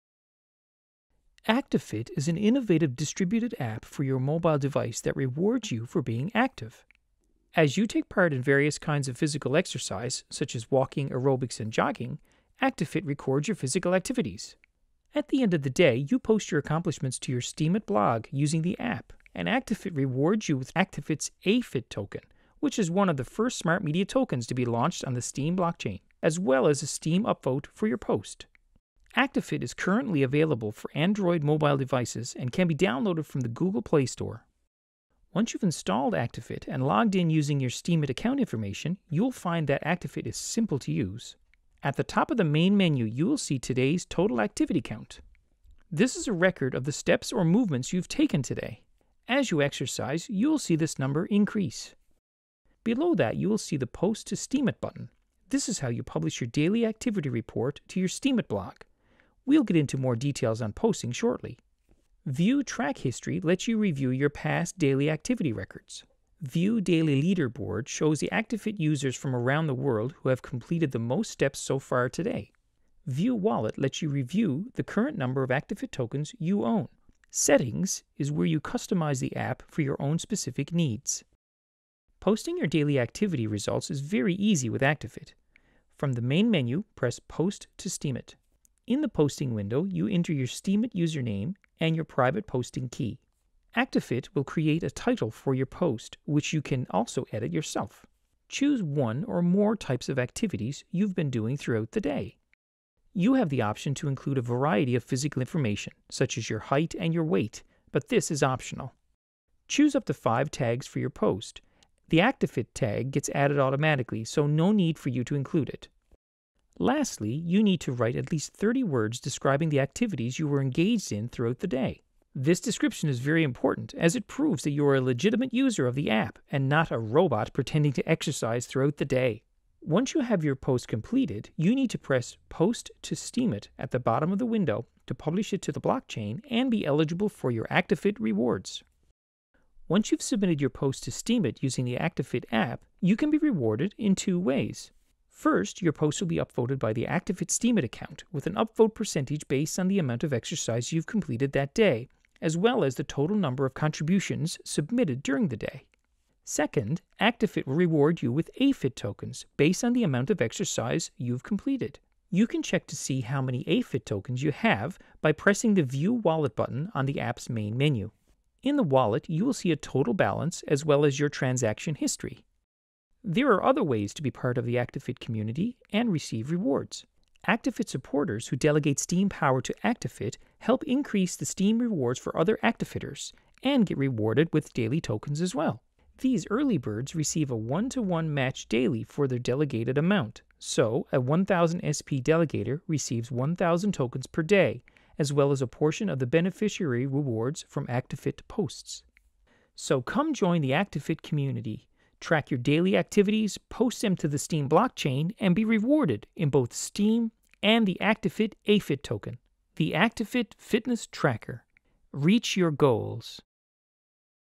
Actifit Fitness Tracker Tutorial
This is a video tutorial for an amazing open source fitness tracker called Actifit. As you use this app to track your daily activities the app rewards you with crypto currency.

You can download this app from the Google Play Store.